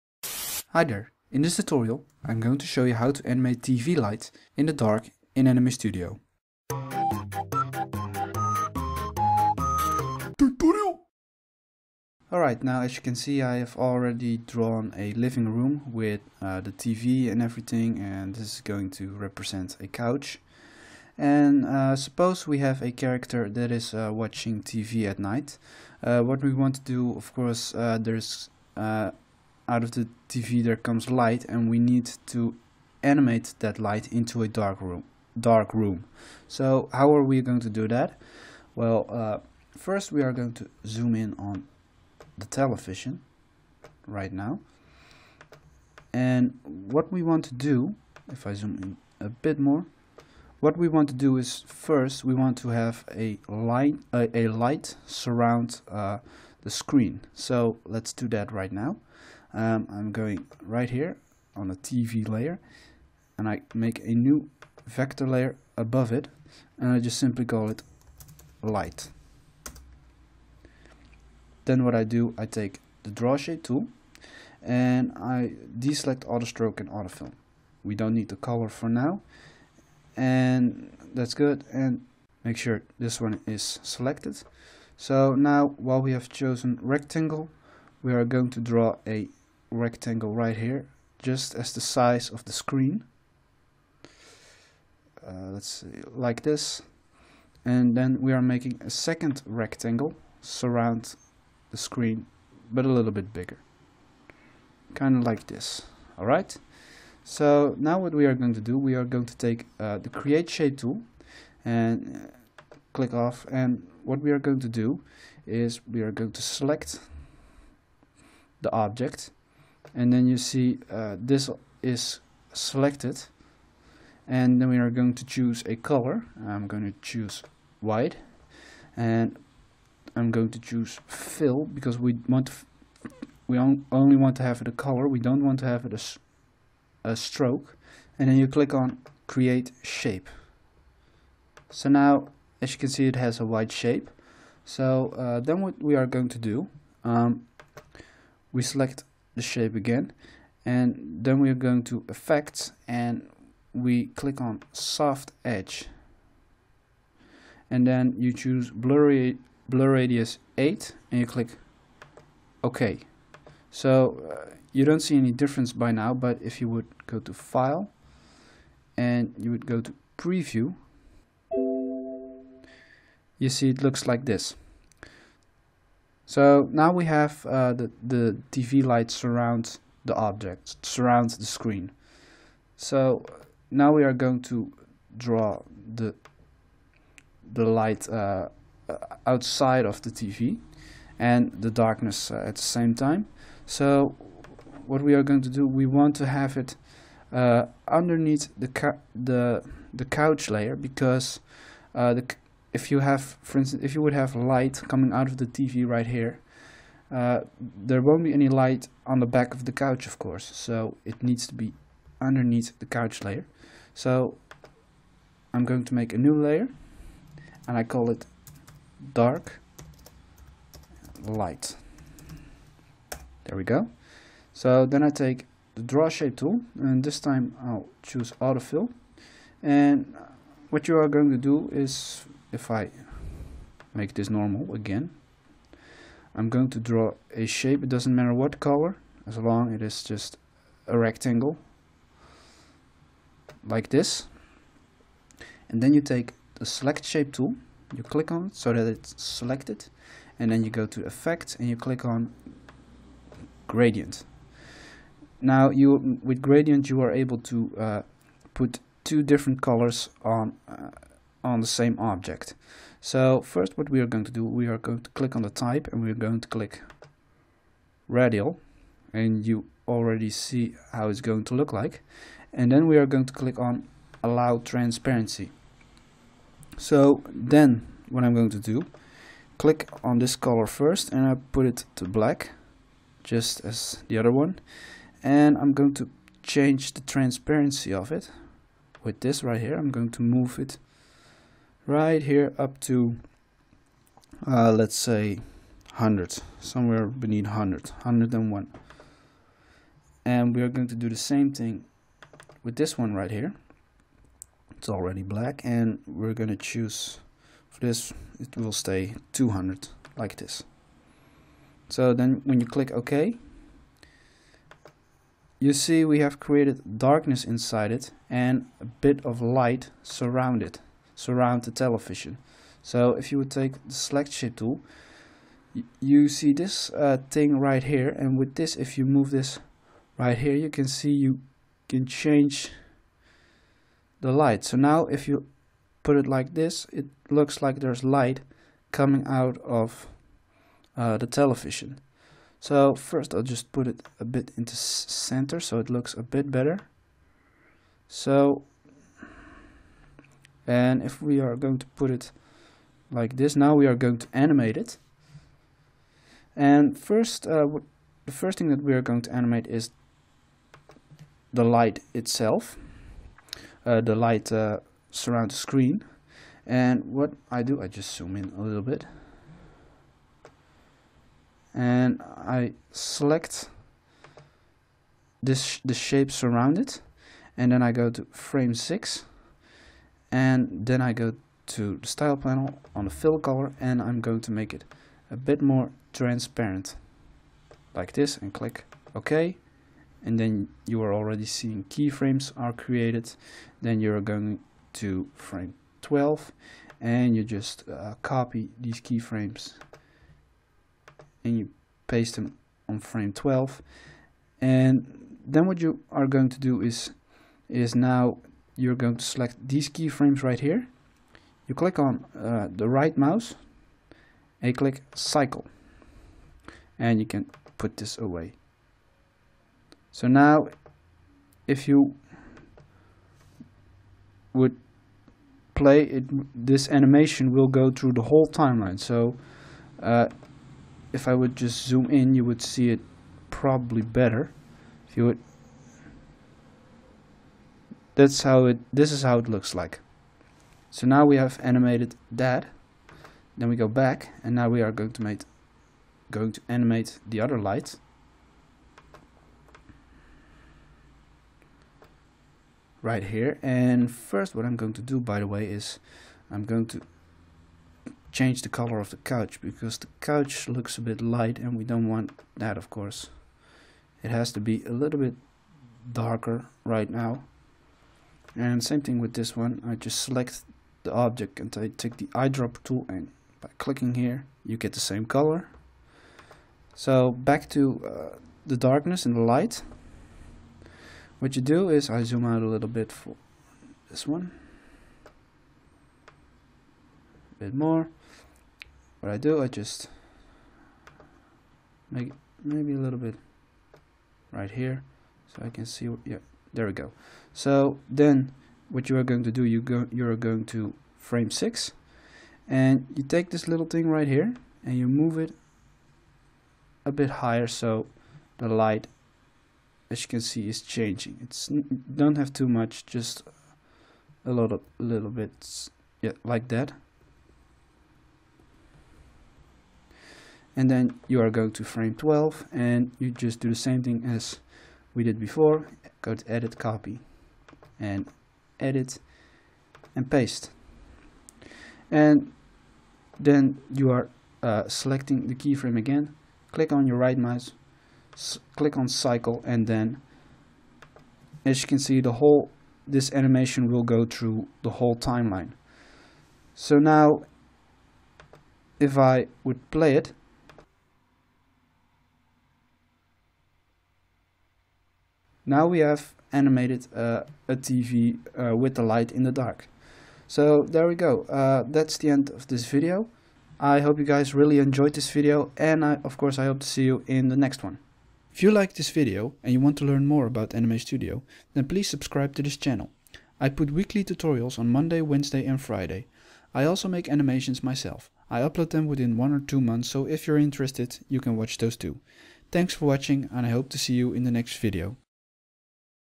How to animate tv light in the dark in Anime Studio - Toons ForYou
In this tutorial, I teach you how to animate tv light in the dark in Anime Studio Pro.